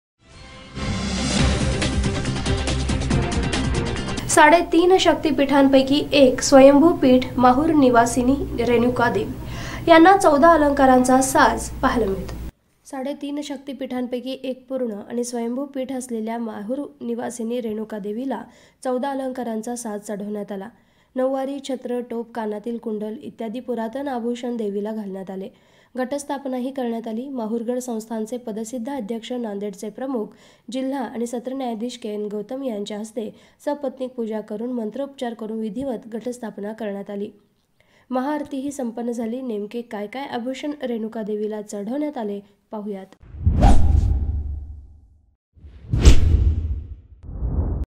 MAHARURGAD | साडेतीन शक्तीपीठांपैकी एक स्वयंभू पीठ माहुरनिवासींनी रेणुका देवीला 14 अलंकाराचा साज
साडेतीन शक्तीपीठांपैकी एक स्वयंभू पीठ माहुरनिवासींनी रेणुका देवीला 14 अलंकाराचा साज